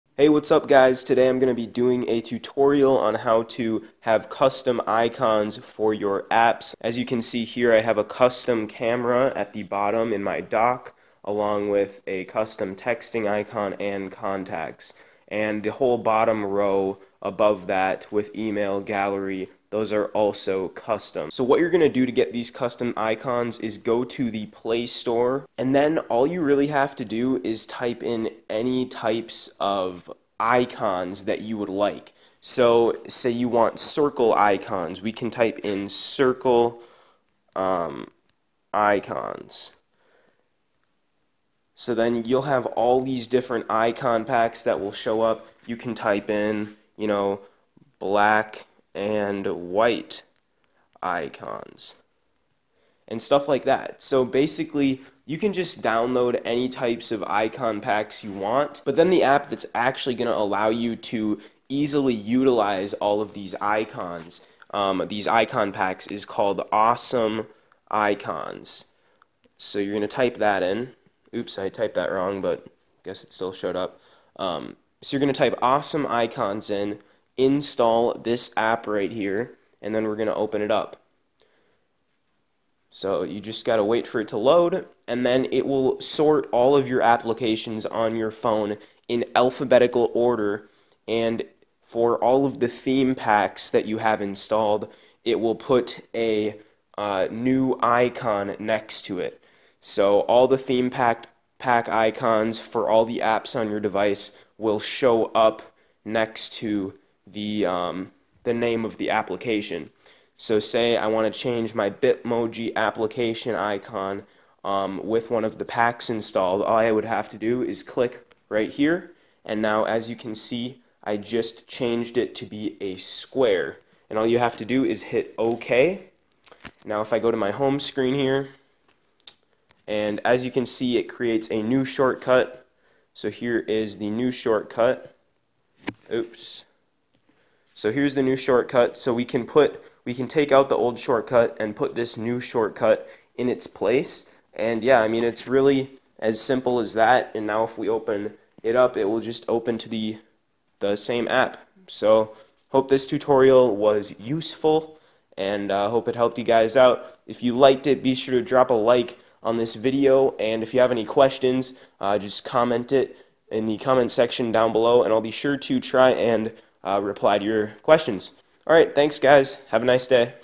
How to customize/change your app icons on ANY android device!
This video shows you how to change your application icons to any style/theme you want on any android device in a very easy to understand way. An LG G4 device was used in this video.